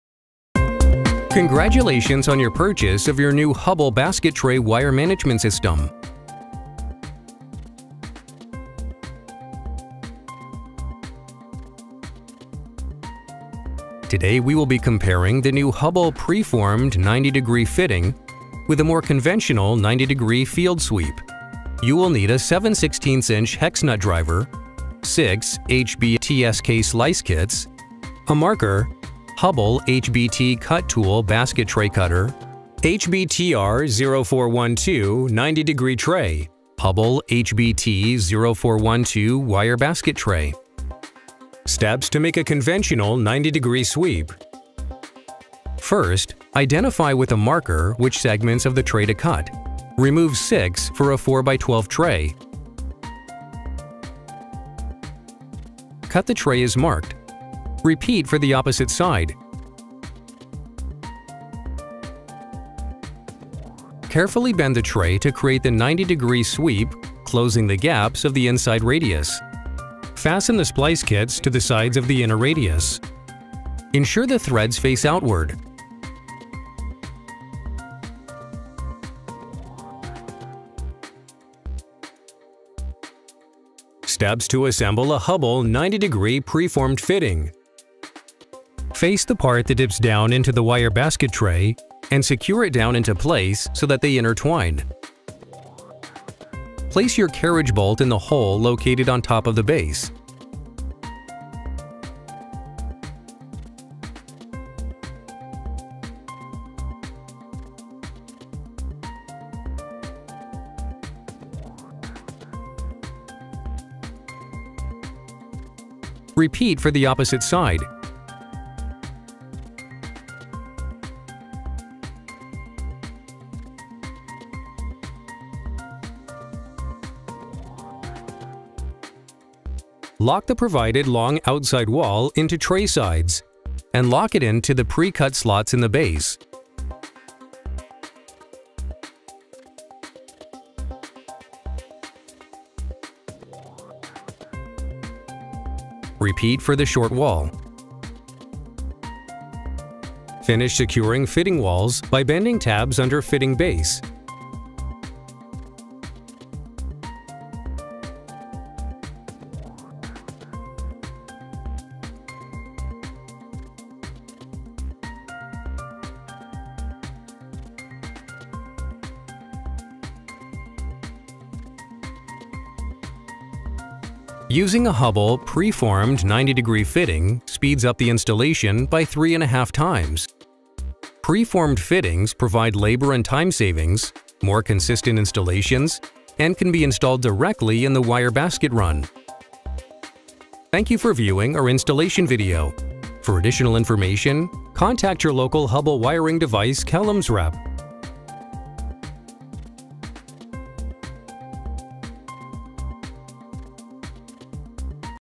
Hubbell Wiring - Wire Basket Tray Pre Formed Fittings
Hubbell Wiring Device-Kellems Develops Innovative Wire Basket Solutions

Several new products have been added to the Wire Basket Tray System product line. This comprehensive product portfolio offers overhead and raised-floor applications, from data centers to highly corrosive environments, such as food processing plants. Many labor-saving options are also available such as no-splice supports and broad offering of pre-formed fittings.

Wire Basket Tray Systems are available in several variations, including flat cross-wire and round cross-wire designs. Finishes include pre-galvanized and electroplated zinc for commercial and controlled interior environments, stainless steel for industrial and outdoor environments requiring corrosion resistance, painted finishes for data centers and other controlled environments.

Many labor-saving options are also available, including:

No-splice supports that decrease material costs by reducing the number of splice kits required for installation
Pre-formed fittings that eliminate the need for field cuts by attaching at intersection tees or junctions
Painted trays with factory-installed masking that reduces onsite paint removal time
Jobsite location palletization and markings so contractors can easily find what they need
New Wire Basket Tray System accessories include grounding split bolts for supplemental grounding, cable drop-outs that reduce pinch points and maintain a radius bend on exiting cables, strut clips to hold the basket tray down, and mounting hardware.